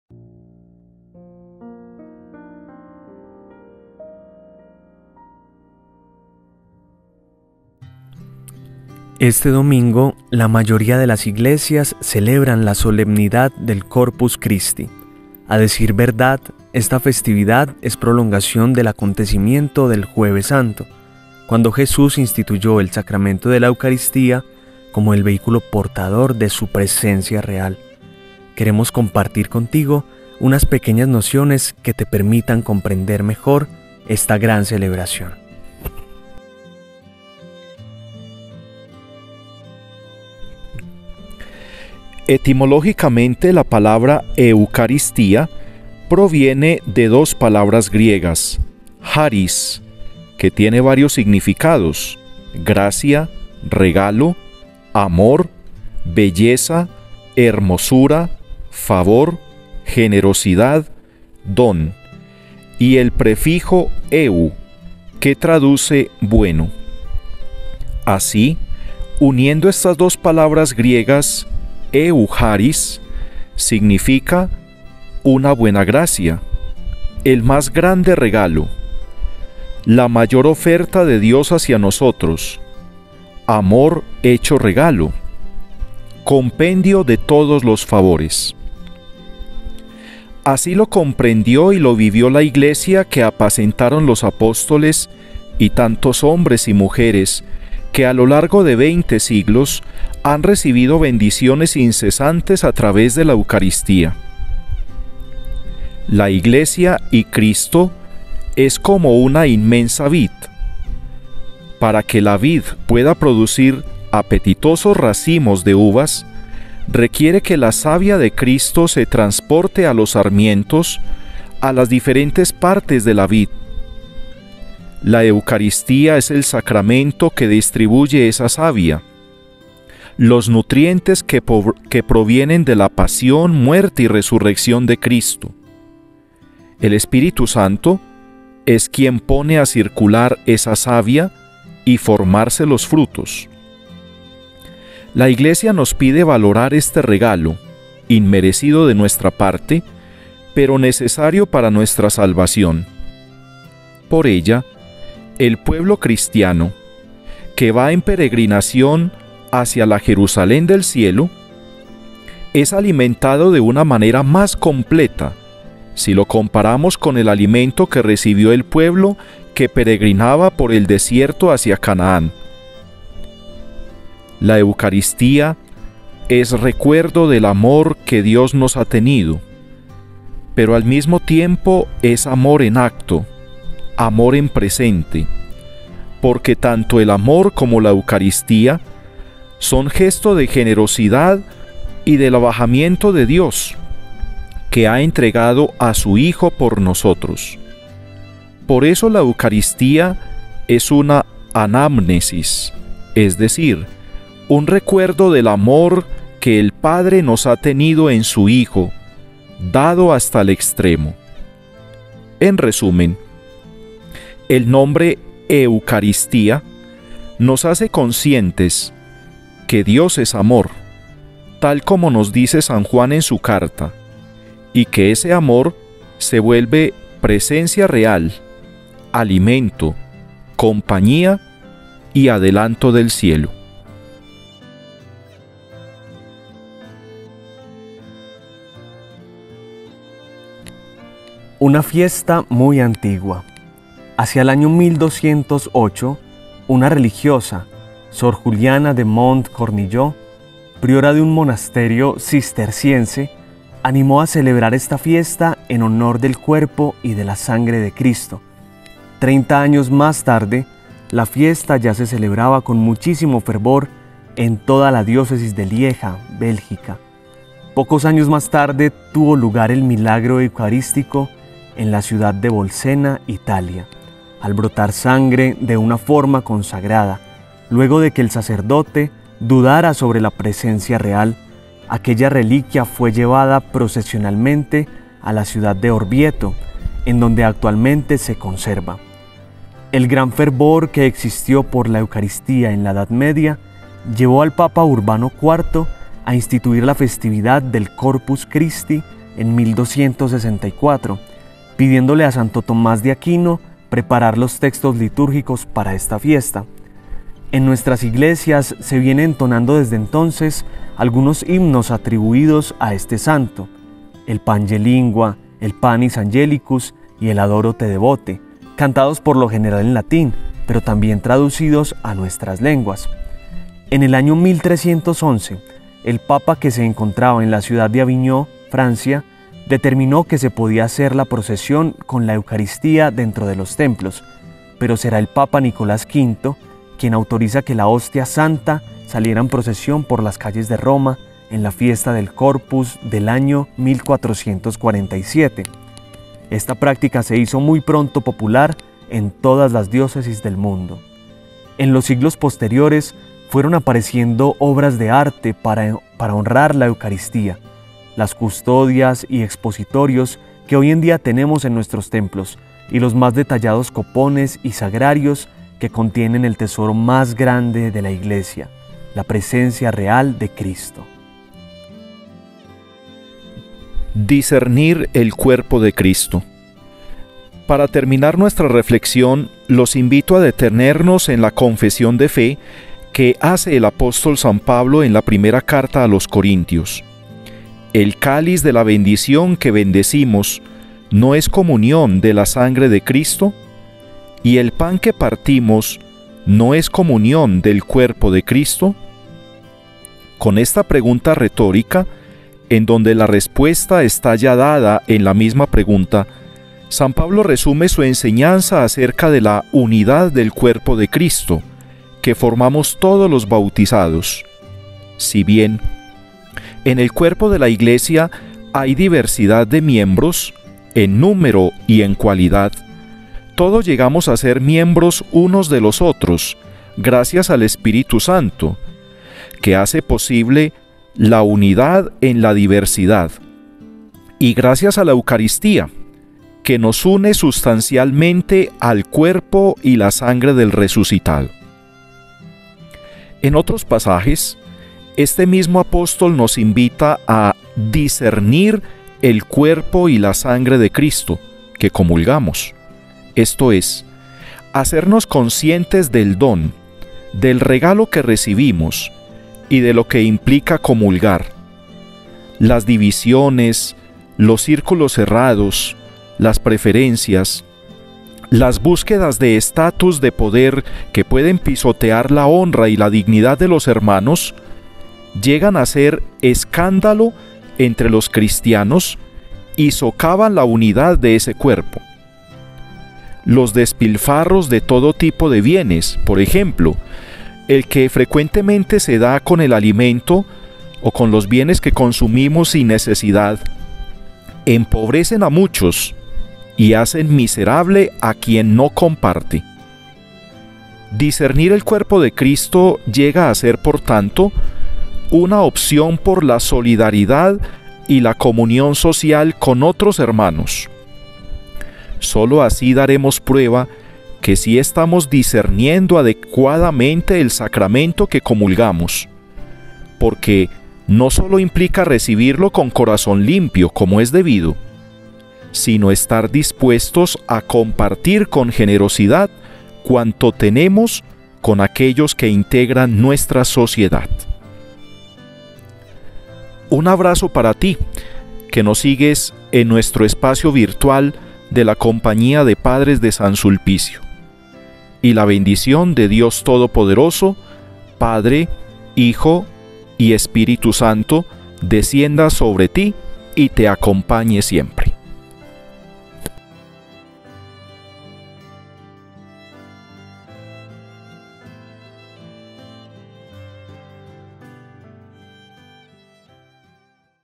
CORPUS CHRISTI - FIESTA del Cuerpo y Sangre de Cristo. 📖 Lectio Divina Evangelio junio 11 de 2023
Este domingo, la mayoría de las iglesias celebran la solemnidad del Corpus Christi. A decir verdad, esta festividad es prolongación del acontecimiento del jueves santo, cuando Jesús instituyó el sacramento de la Eucaristía como el vehículo portador de su presencia real. Queremos compartir contigo unas pequeñas nociones que te permitan comprender mejor esta gran celebración.

En aquel tiempo, Jesús dijo a los judíos: "Yo soy el pan vivo que ha bajado del cielo; el que coma de este pan vivirá para siempre. Y el pan que yo les voy a dar es mi carne para que el mundo tenga vida".

Entonces los judíos se pusieron a discutir entre sí: "¿Cómo puede éste darnos a comer su carne?"

Jesús les dijo: "Yo les aseguro: Si no comen la carne del Hijo del hombre y no beben su sangre, no podrán tener vida en ustedes. El que come mi carne y bebe mi sangre, tiene vida eterna y yo lo resucitaré el último día.

Mi carne es verdadera comida y mi sangre es verdadera bebida. El que come mi carne y bebe mi sangre, permanece en mí y yo en él. Como el Padre, que me ha enviado, posee la vida y yo vivo por él, así también el que me come vivirá por mí.

Éste es el pan que ha bajado del cielo; no es como el maná que comieron sus padres, pues murieron. El que come de este pan vivirá para siempre".

El Padre Jorge Castellanos, PSS, comparte con nosotros el EvangelioDeHoy​​​​ domingo 11 de junio de 2023 📘 Celebración del Corpus Christi, cuerpo y sangre de Cristo.